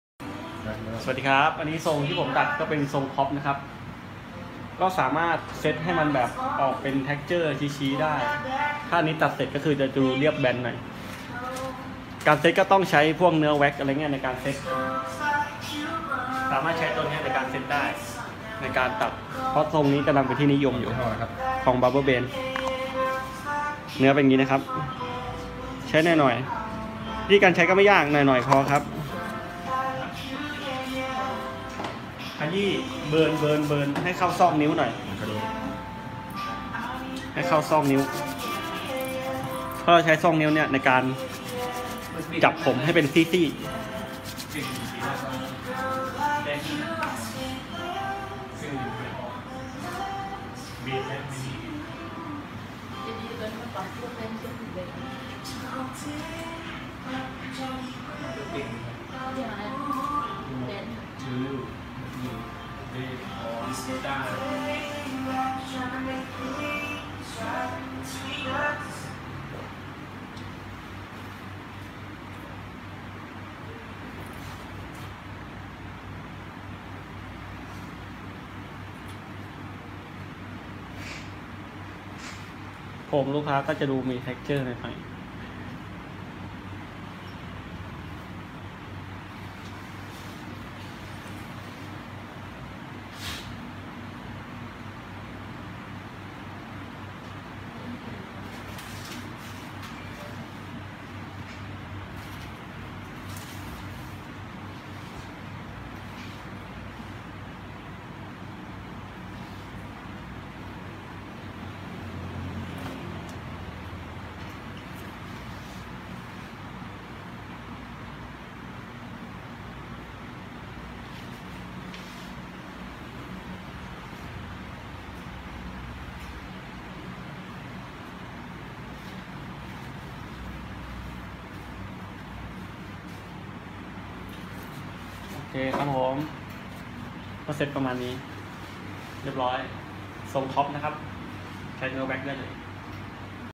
เซตผมด้วย Super wax ช่างฮารีส จากหาดใหญ่
ตัดผมได้ทรงผม ออกมาว่าสวยแล้ว
เซ็ททรงเสร็จ เด็ดกว่าเดิมไปอี๊กกกกกกกกกกก
ช่างฮารีส จากหาดใหญ่
เซ็ททรงกันให้ดูแบบชัดๆ ด้วย บาร์เบอร์ เบรน